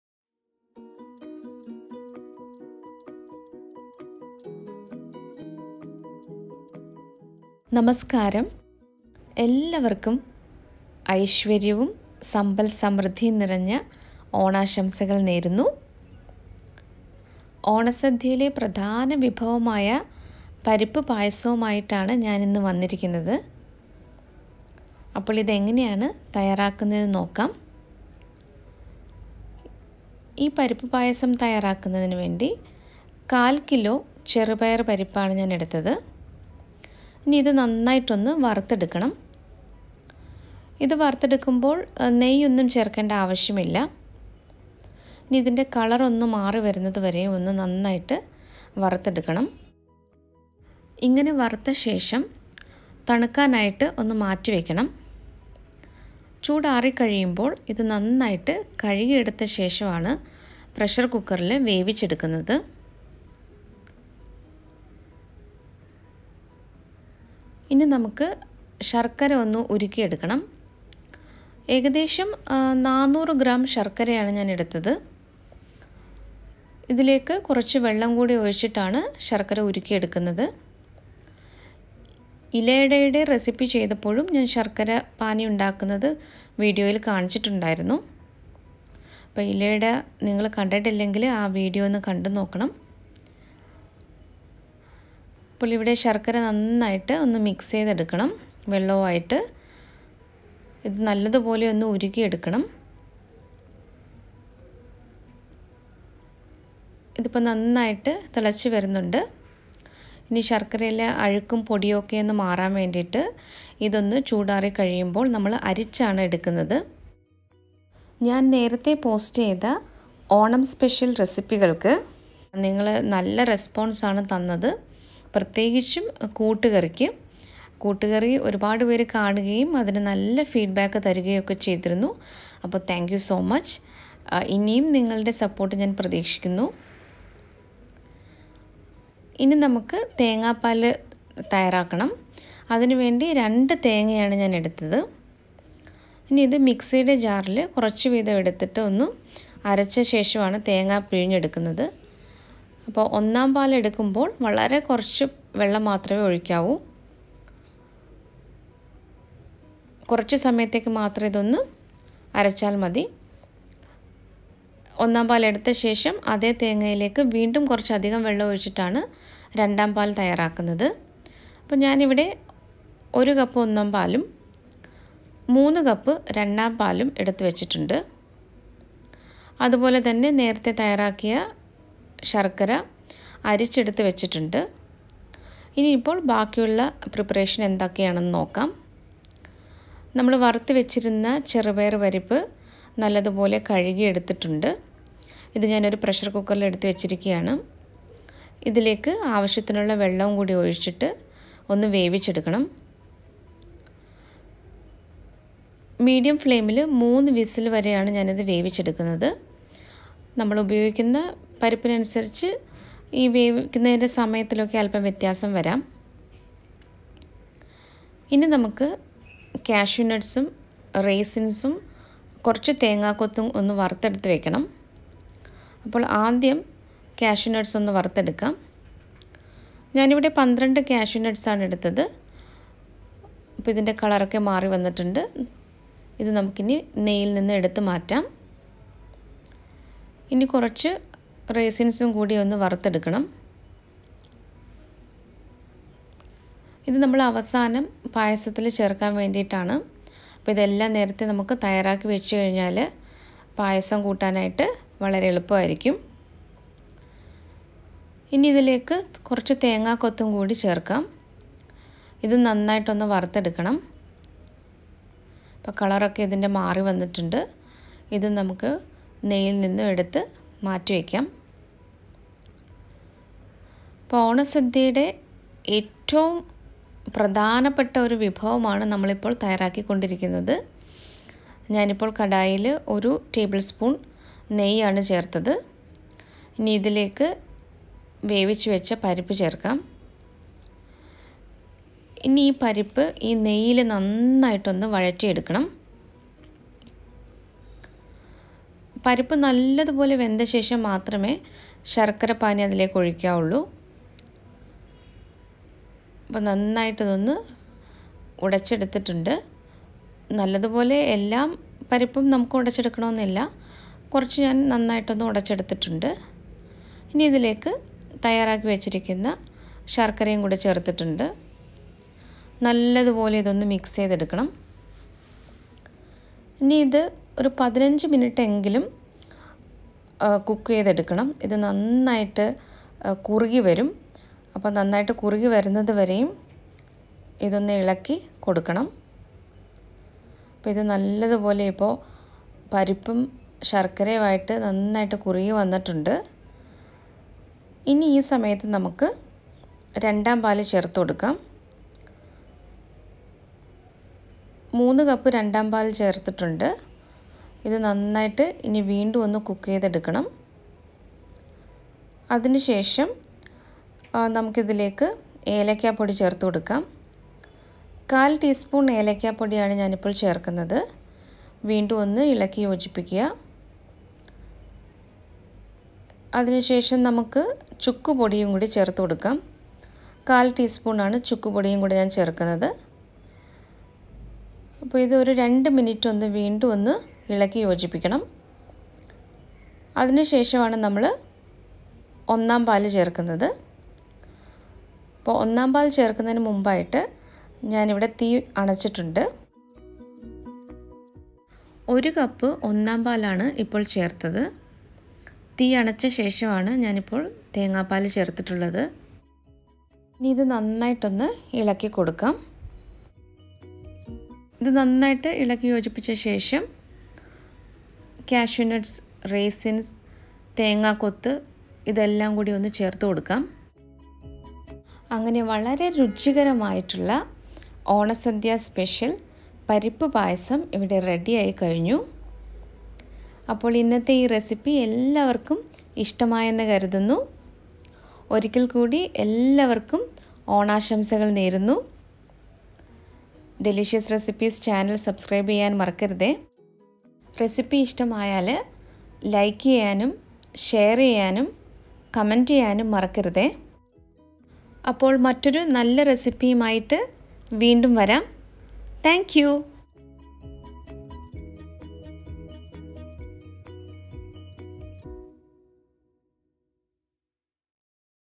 ഓണസദ്യ സ്പെഷ്യൽ പരിപ്പ് പായസം || Onam Special Parippu Pradhaman || DELICIOUS RECIPES
INGREDIENTS

Moong dal -1/4 kg
Jaggery-400 g
Water-
Coconut-2
Thick Coconut milk-1 cup
Thin Coconut milk-3 cups
Cashew nuts-12
Raisins-
Coconut pieces-
Ghee-